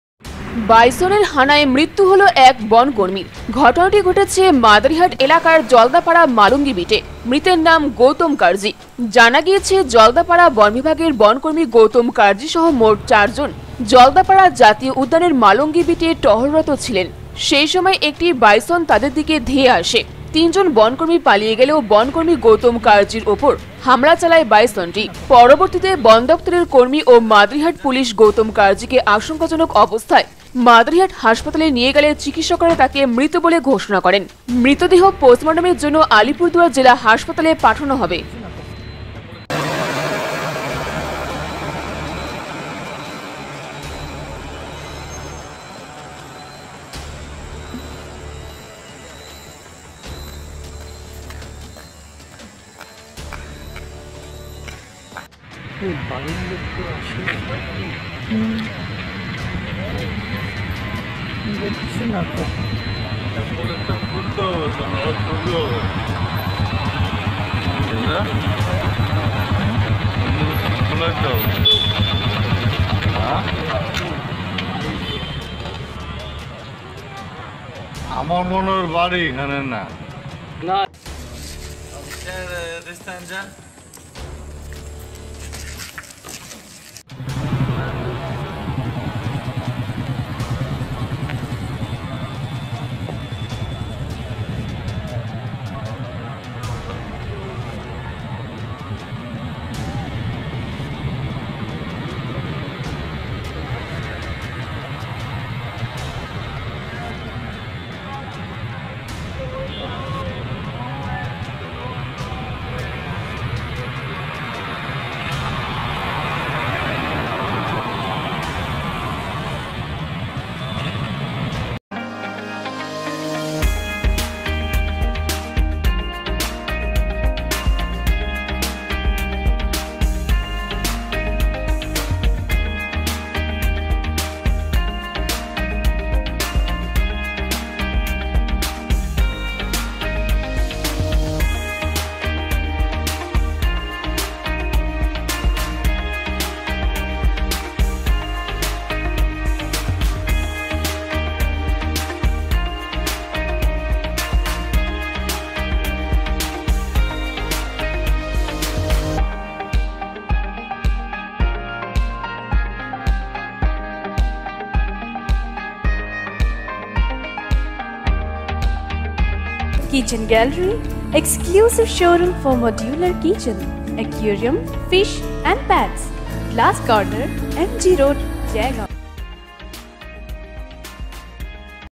Jaldapara : বাইসনের হানায় মৃত্যু হল এক বনকর্মীর, ঘটনা আলিপুরদুয়ার জেলার মাদারিহাট এলাকায় জলদাপাড়া মা
Jaldapara : বাইসনের হানায় মৃত্যু হল এক বনকর্মীর, ঘটনা আলিপুরদুয়ার জেলার মাদারিহাট এলাকায় জলদাপাড়া মালঙ্গী বীটে